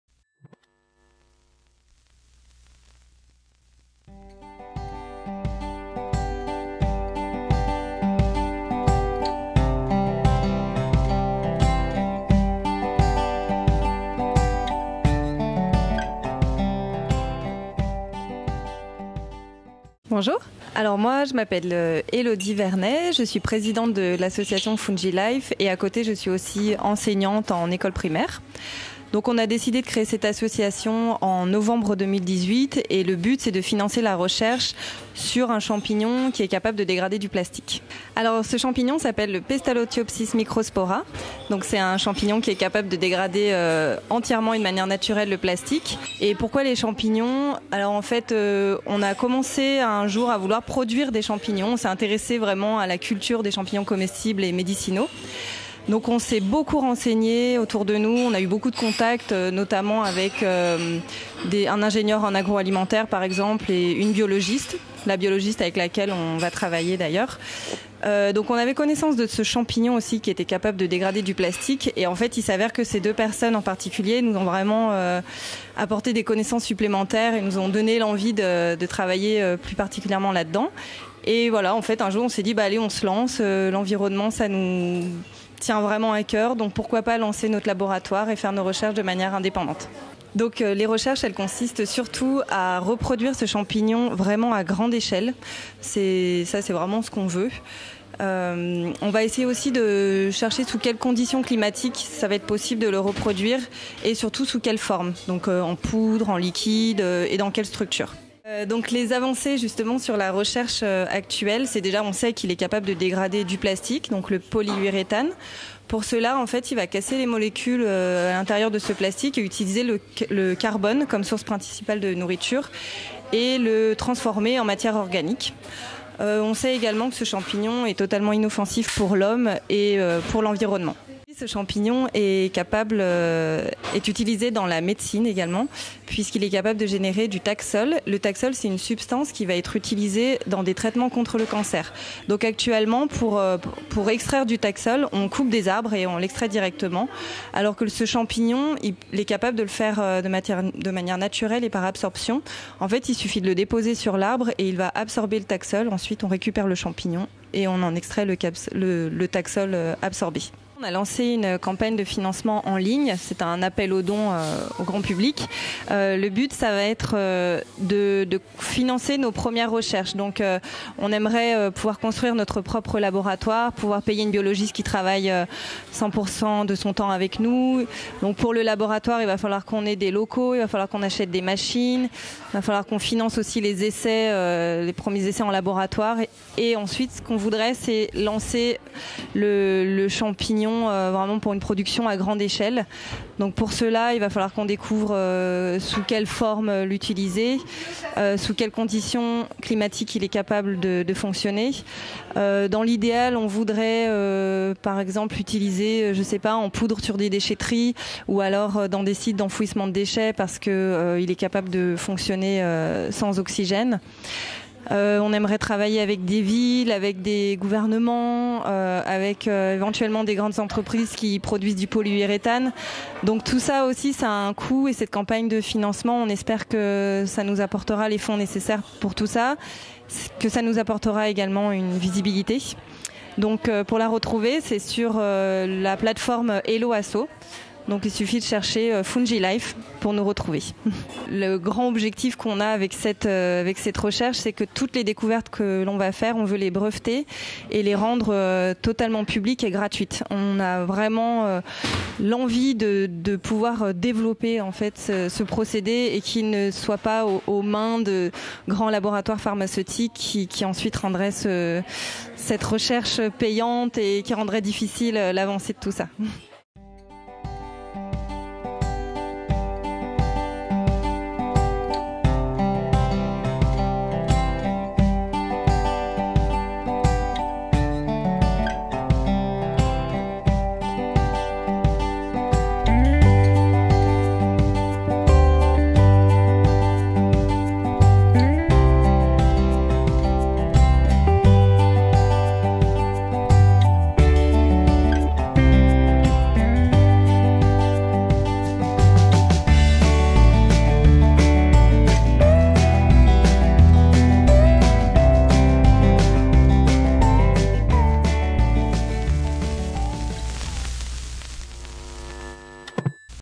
Couleur Café présente : « Pestaliotopsis Microspora », le champignon dévoreur de plastique !
Fungi Life est une association Lyonnaise dédiée à la recherche sur le champignon Pestaliotopsis Microspora. Ce champignon a pour propriété très particulière de se nourrir de polyuréthane ; le développement de son utilisation peut permettre un « recyclage naturel » de ce type de plastique.
Retrouvez-nous sur : ccc-media.fr
Notre FB : CCC Média